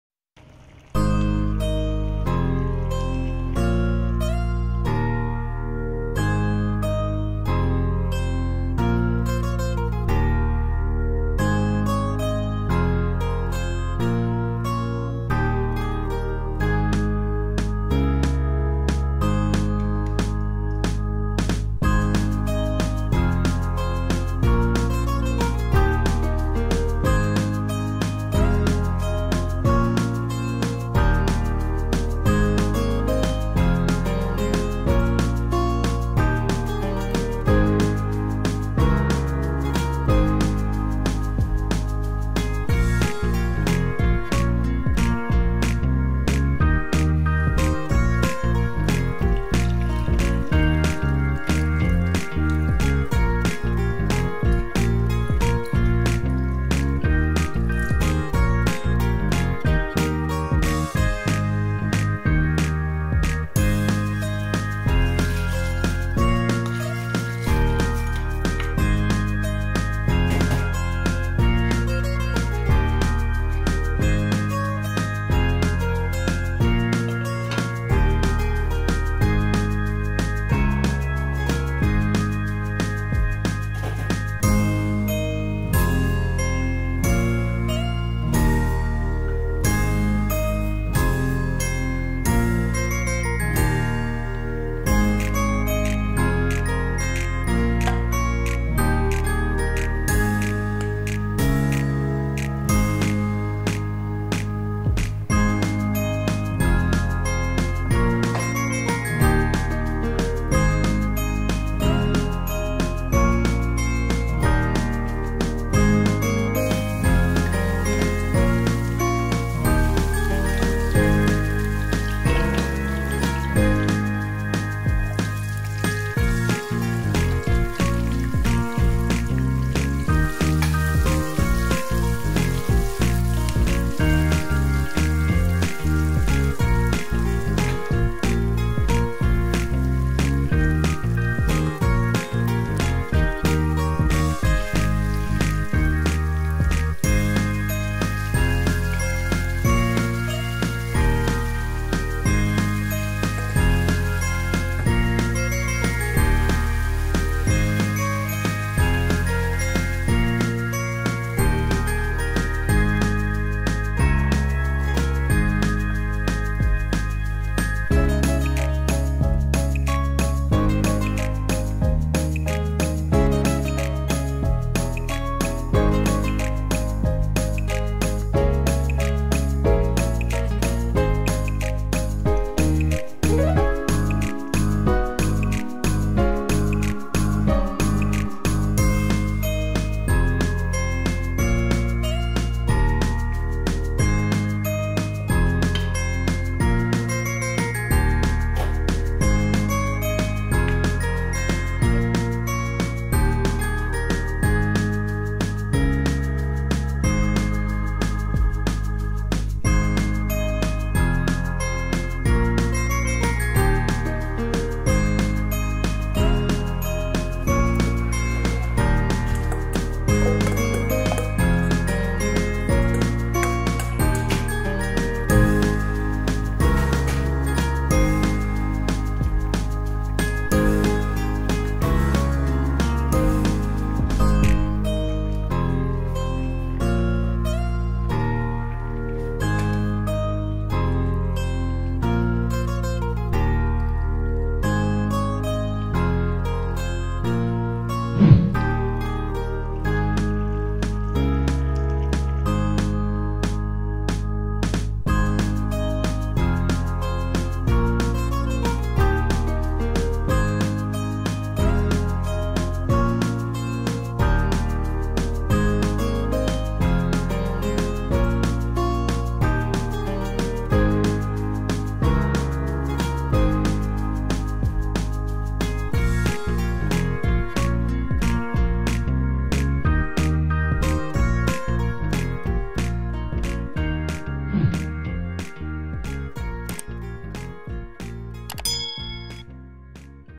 PORK PATA WITH BLACK BEANS AND MALUNGGAY LEAVES | FOR BEGINNERS | PANLASANG FILIPINO
PORK PATA WITH BLACK BEANS AND MALUNGGAY | FOR BEGINNERS | PANLASANG FILIPINO

Ingredients:
1 kg Pork Pata
1 cup Black Beans
1 piece Pork broth Cubes
2 cups Malunggay Leaves
1 thump-size Ginger
4-5 cups of water
Fish Sauce and Black Pepper to taste

Enjoy watching and cooking!

PINOY FOOD TV